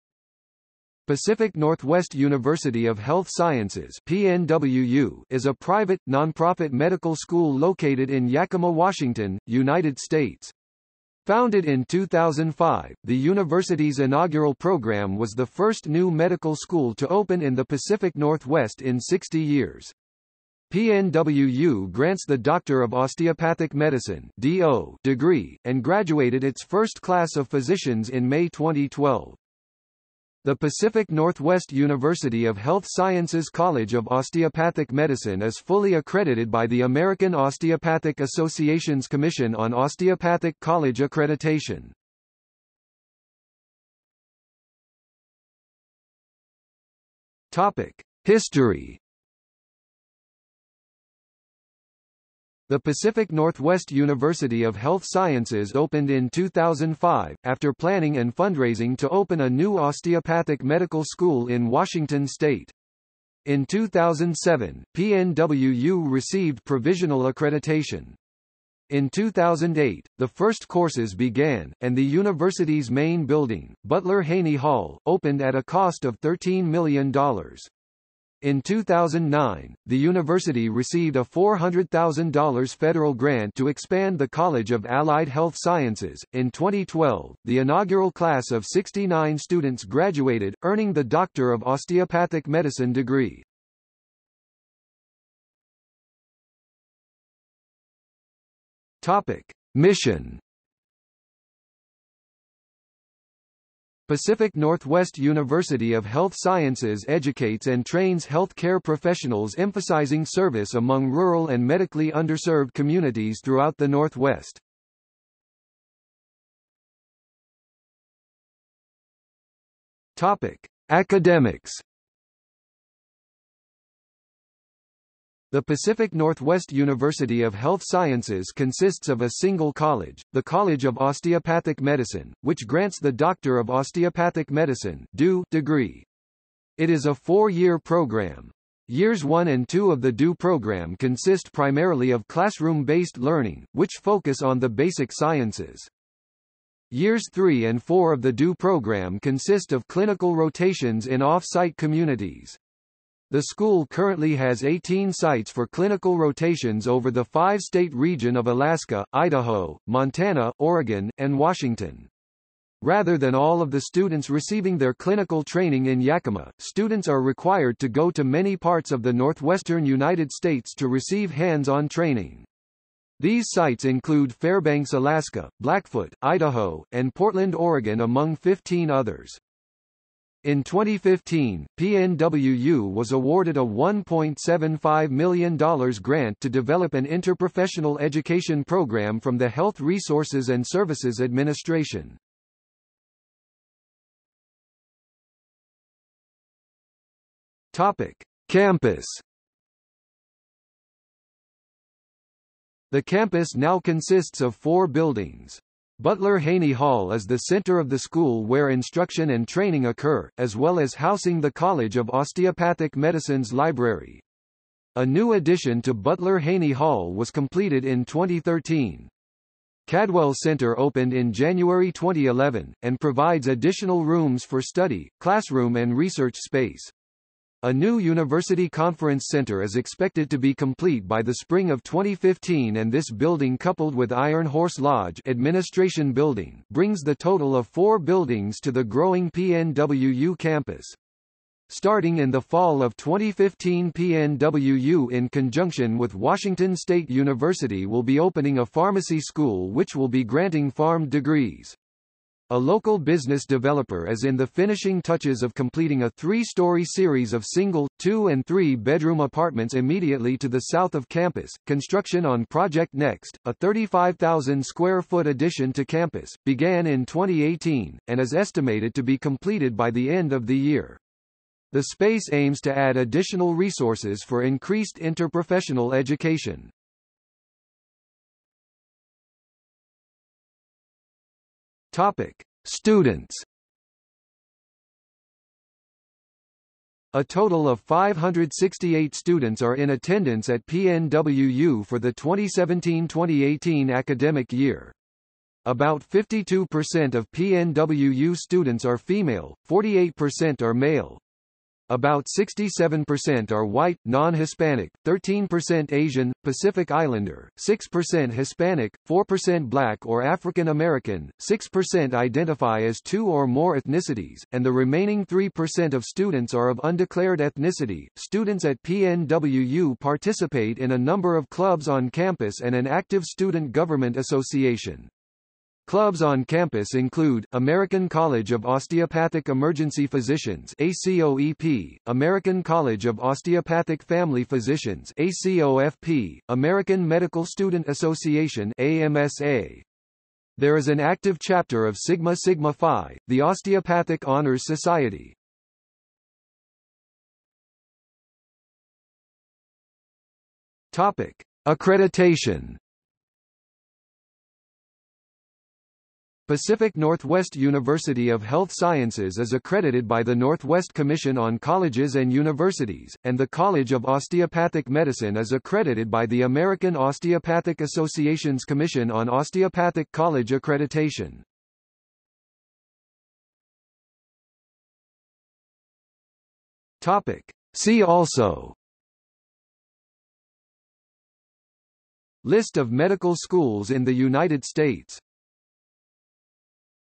Pacific Northwest University of Health Sciences | Wikipedia audio article
00:00:43 1 History
00:01:35 2 Mission
00:01:56 3 Academics
00:03:14 4 Campus
00:04:38 5 Students
00:05:47 6 Accreditation
00:06:13 7 See also

- Socrates

SUMMARY
Pacific Northwest University of Health Sciences (PNWU) is a private, non-profit medical school located in Yakima, Washington, United States.  Founded in 2005, the university's inaugural program was the first new medical school to open in the Pacific Northwest in sixty years.  PNWU grants the Doctor of Osteopathic Medicine (D.O.) degree, and graduated its first class of physicians in May 2012.
The Pacific Northwest University of Health Sciences College of Osteopathic Medicine is fully accredited by the American Osteopathic Association's Commission on Osteopathic College Accreditation.